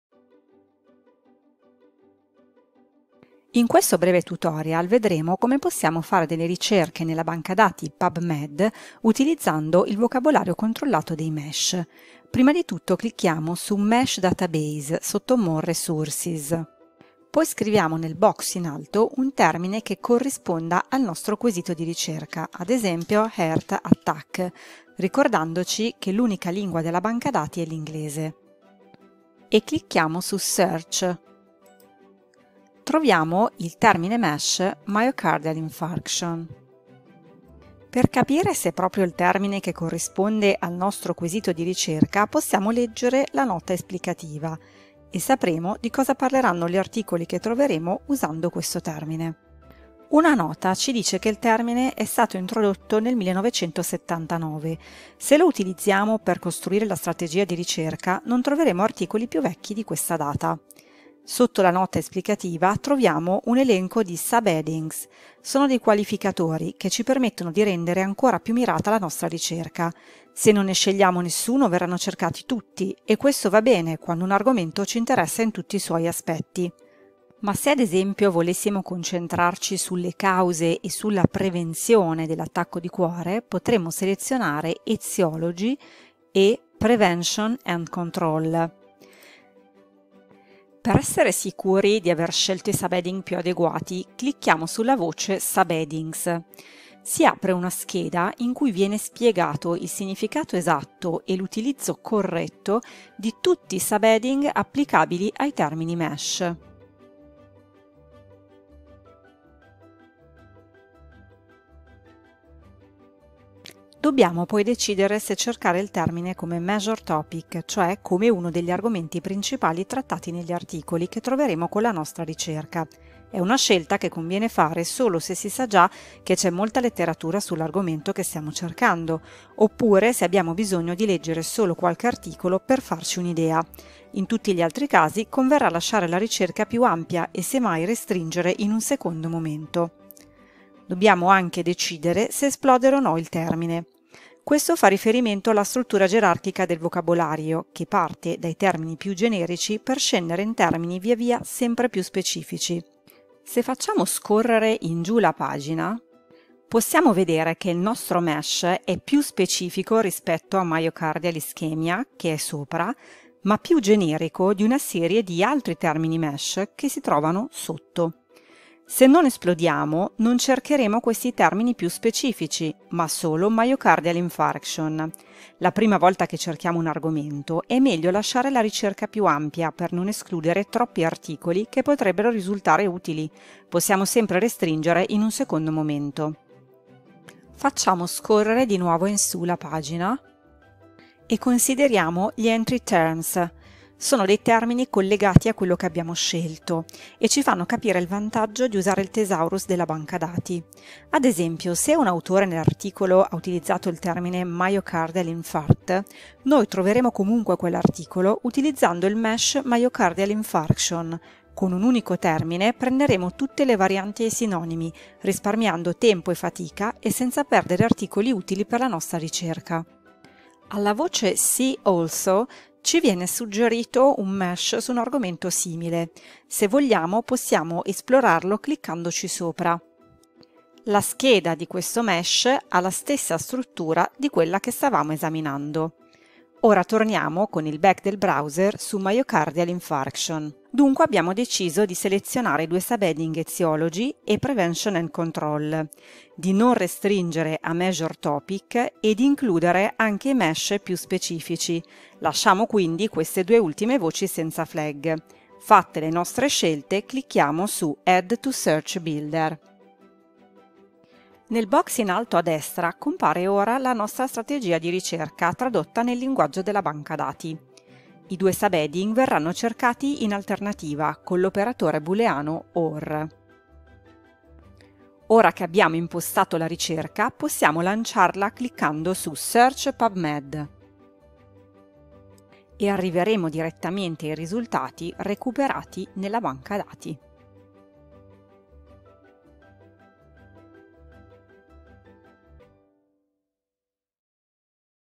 PubMed: ricerca con i MeSH
come effettuare in Pubmed una ricerca con i termini del thesaurus MeSH